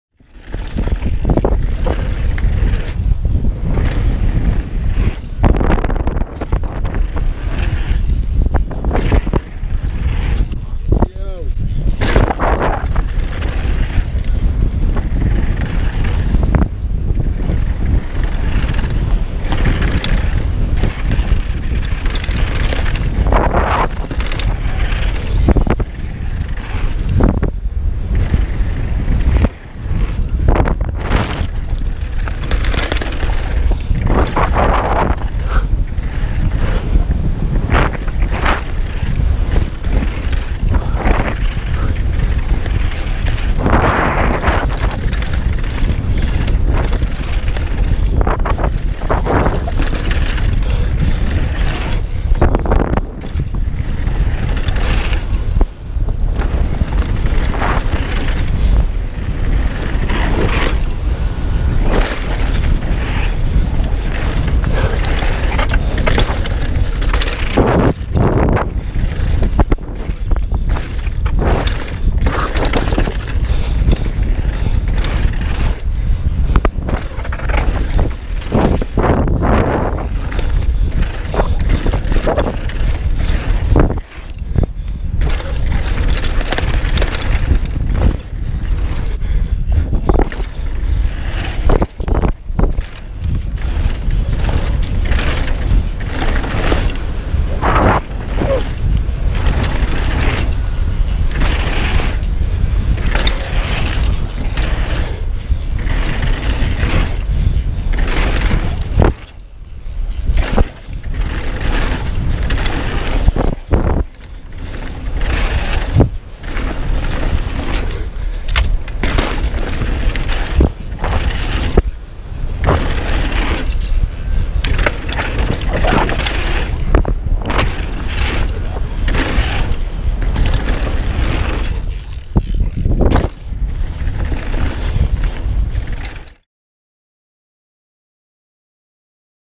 Gopro / Vincent Tupin / Vink Line Chatel
A run down the Vink line in Chatel Bikepark.